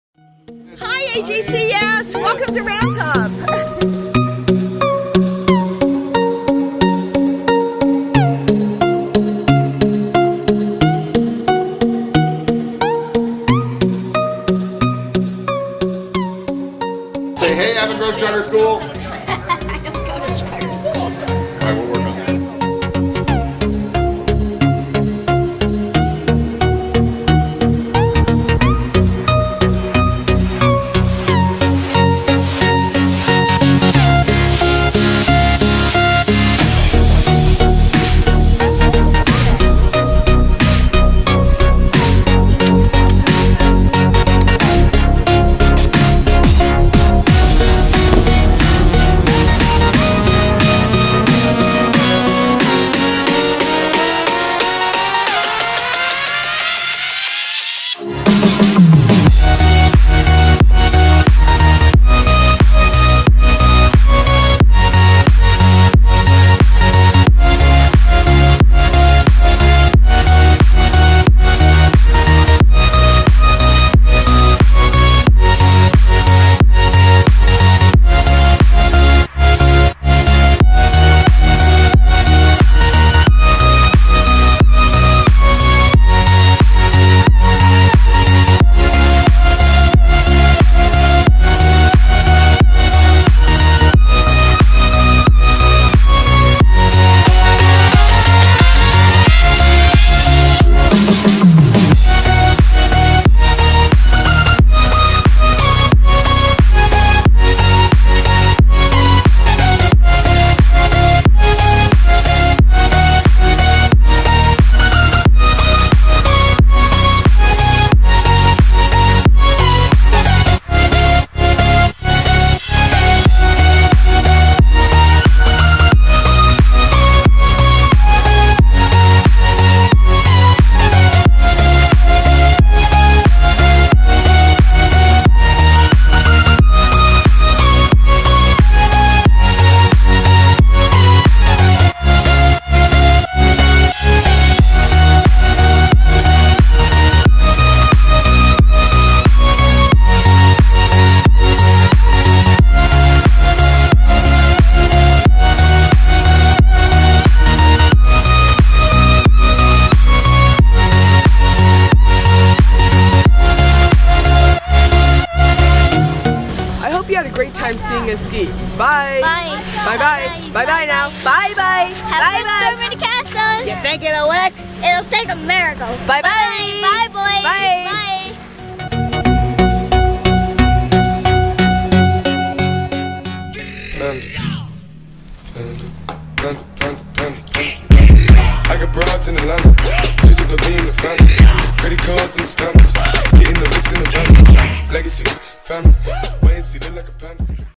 Tommy Tyree Skiing 2017
Tommy and Lauren Tyree Skiing at Roundtop Mountain Resort in Pennsylvania.  Kudos to the RMR Park crew for building such awesome features.  Shout out to AGCS - come skiing with us soon!